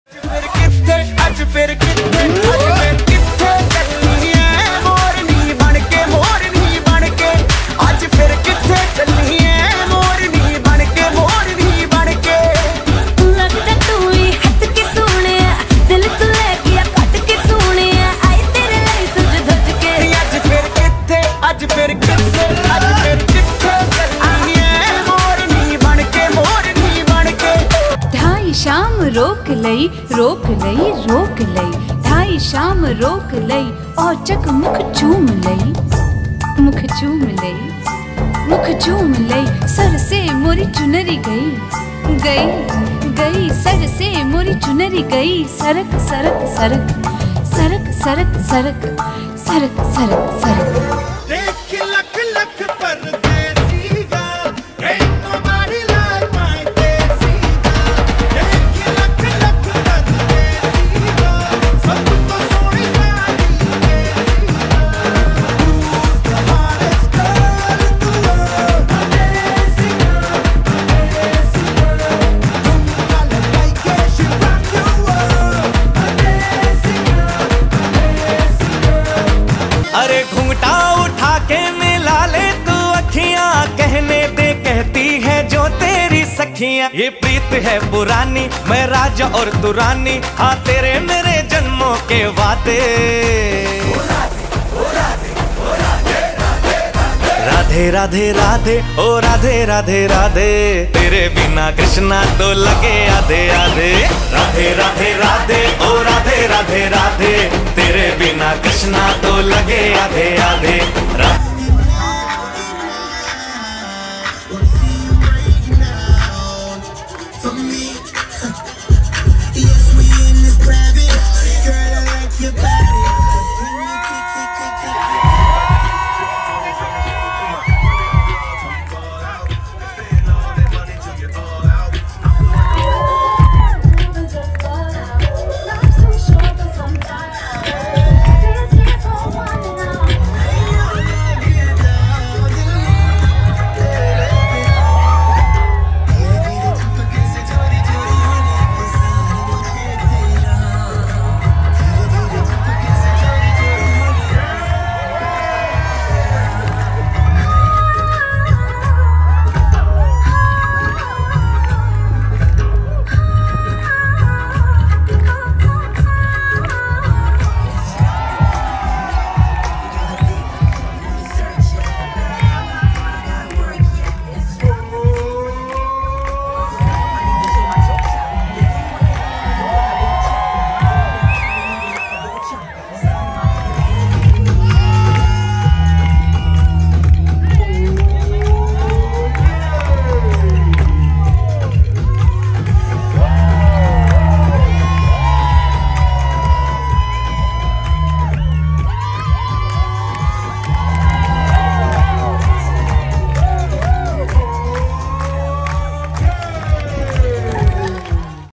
Bollywood dance in paris // festival of dance in paris
1 km of danse is a dance festival organized by center national of dance in paris to bring together different associations of dance on a common platform to share their culture and art with others .. Triwat dance school participated in this festival .. presented different forms of bollywood dance and also made public dance on the foot tapping rythms of Bollywood music.